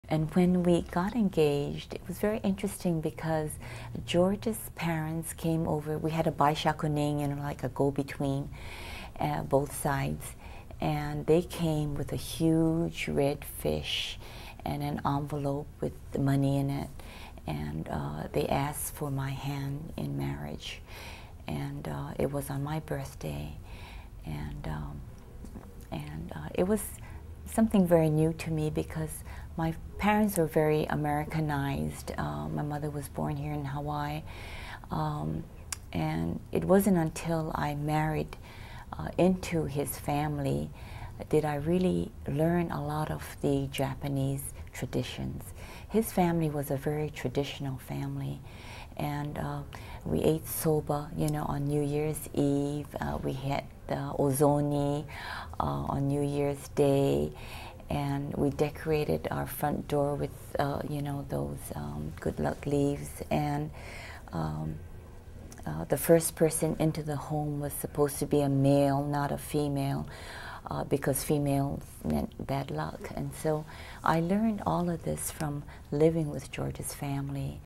Jean Hayashi Ariyoshi #2: Getting Married
Date: December 15, 2003
Location: Hawai`i, US
Interviewer: Lisa Itagaki
Contributed by: Watase Media Arts Center, Japanese American National Museum.